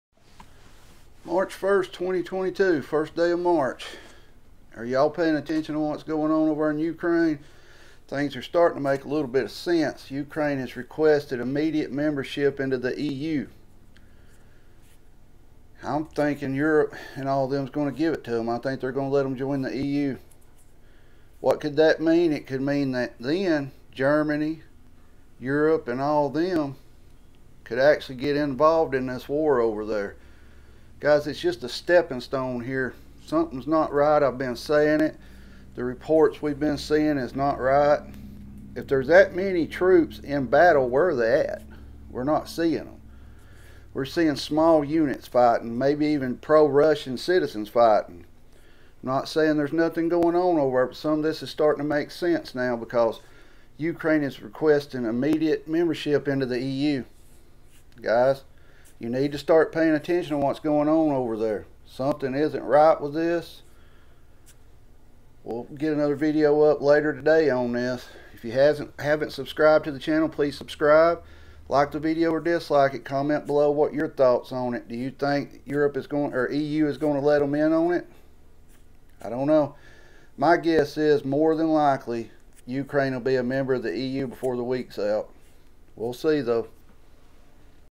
SHTF UKRAINE REQUEST IMMEDIATE ENTRY INTO THE EU.
UKRAINE request immediate approval to join the EU if this happens it allows the EU to become physically active in the war in UKRAINE guys pay attention to what is going on something isn't right with all this